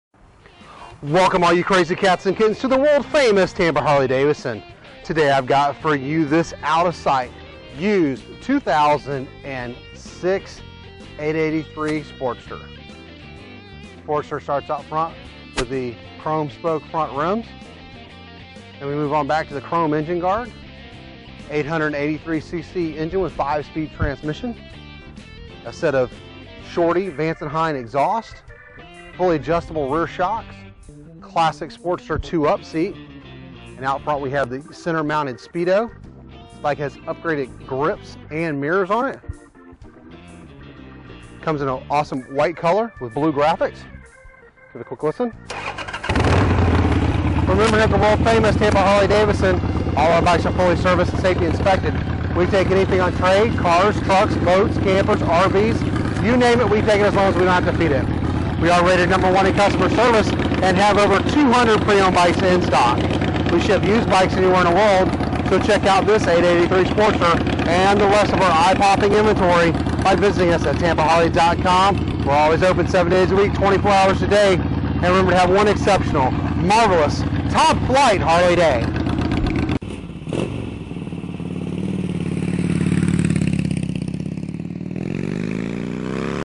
Used Harley-Davidson Sportster 883 for sale motorcycles
Used 2006 Harley-Davidson XL883 Sportster with Vance and Hines exhaust for sale.

Stock# P1486A Vance and Hines Exhaust Upgraded Grips and Mirrors